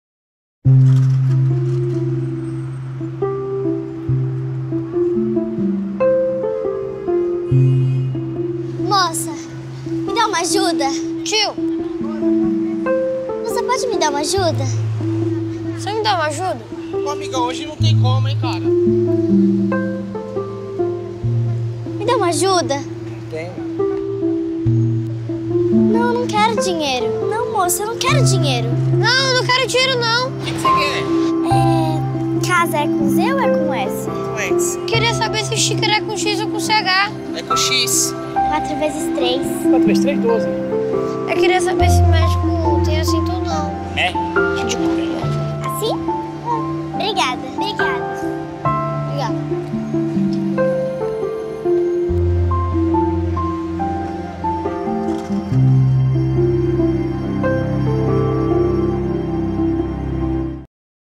Casa do Zezinho: Help
Two young actors, Marjorie Blatt and Victor de Sa, pretended to be underprivileged children and asked for help from people on the street, but it wasn't money they asked for as we see in this ad campaign for the non-profit Brazilian organization Casa do Zezinho.

Credits:
Advertiser: Casa do Zezinho
Title: Help
Product: Institutional
Advertising Agency: AlmapBBDO, Brazil
CCO: Marcello Serpa
ECD: Luiz Sanches
Creative Director: Andre Kassu, Marcos Medeiros, Renato Simões, Bruno Prosperi
Copywriter: Rodrigo Resende
Art Director: Denis Peralta
Producer: Cine
Executive Producer: Raul Doria, Waldemar Tamagno
Director: Felipe Mansur
Photography Director: Marcelo Trotta (Tintin)
Editor: Rodolpho Ponzio, Felipe Mansur
Pos-Production Cine X
Audio Producer: Raw Audio
Soundtrack: Hilton Raw
Maestro: Hilton Raw
RTV: Vera Jacinto, Rafael Motta, Elisa Mello, Thiago Bueno
Account Service: Cristina Chacon, Tássia Massumi
Media: Paulo Camossa, Claudio Knupp
Approval: Dagmar Garroux, Gilson Martins